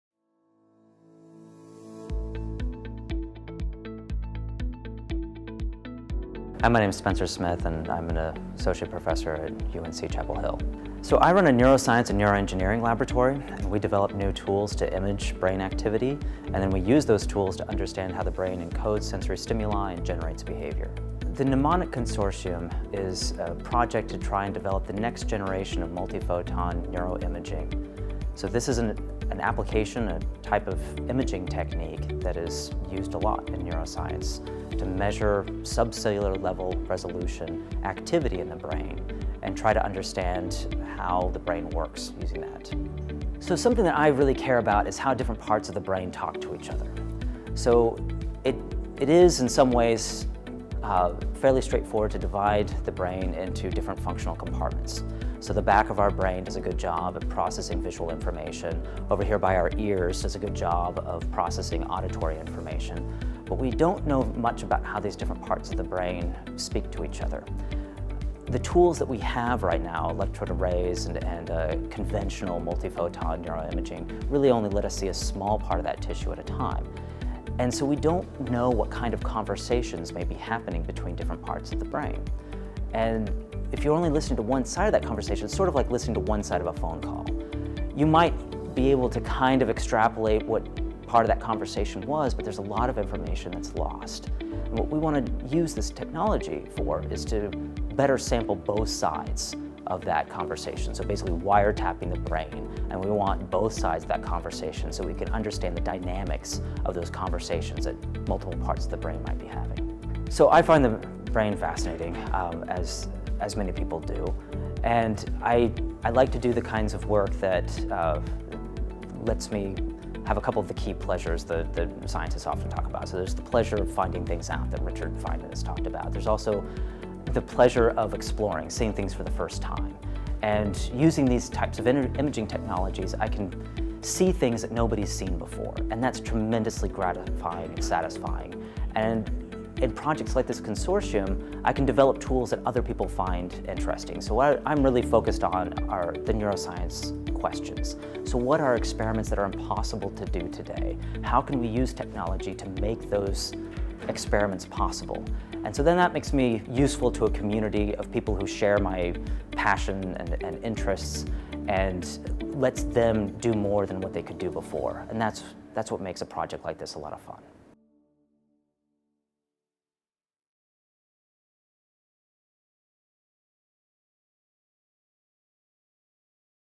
Wiretapping the Brain: Stories From the U.S. BRAIN Initiative
What scientists can learn about the brain is often limited by the tools they have at their disposal. New and improved tools could make previously impossible experiments possible, revealing new insights about the brain. Neuroscientist Spencer Smith of the University of North Carolina, Chapel Hill is hoping to make that a reality. Backed by a U.S. BRAIN Initiative grant from the National Science Foundation, Smith leads the NEMONIC (Next generation MultiphOton NeuroImaging Consortium) project, which aims to develop state-of-the-art imaging tools and make them available to the neuroscience community.